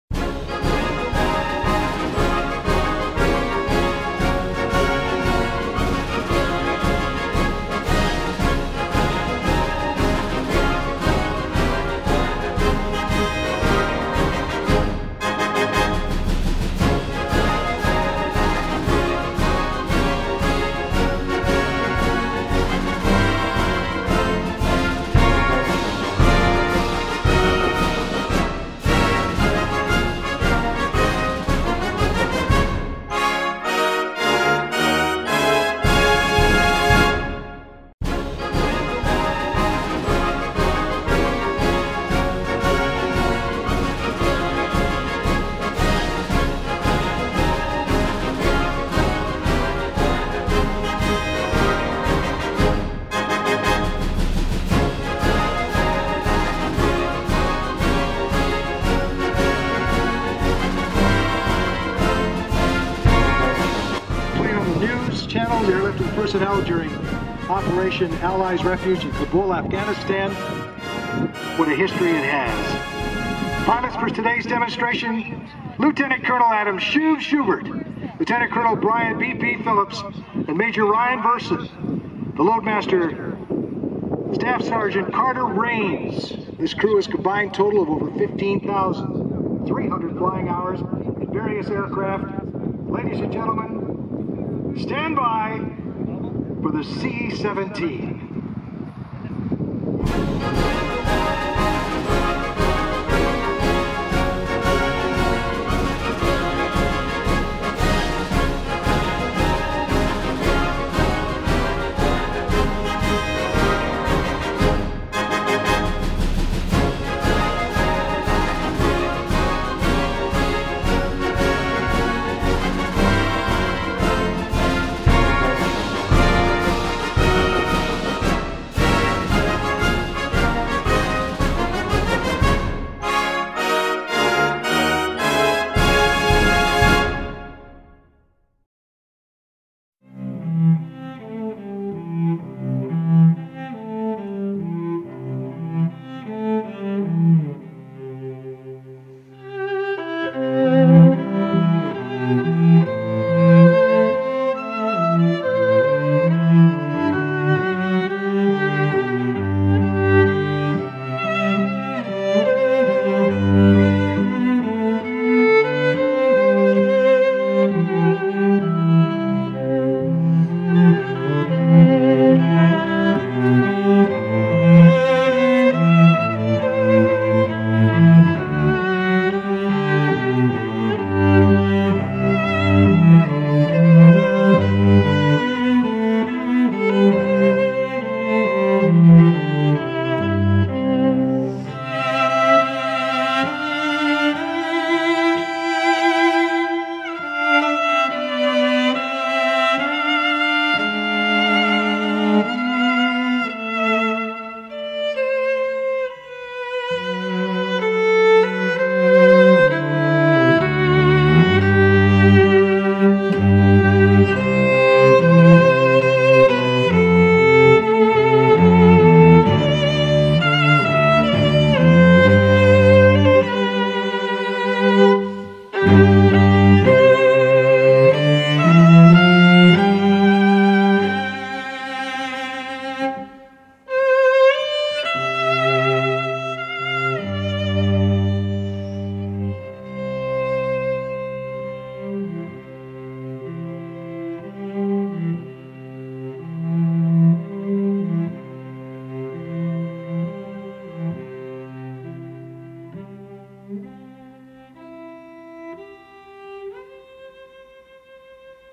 Selfridge Air Show 2022
Selfridge Air Show 2022, featuring the C17 Globemaster.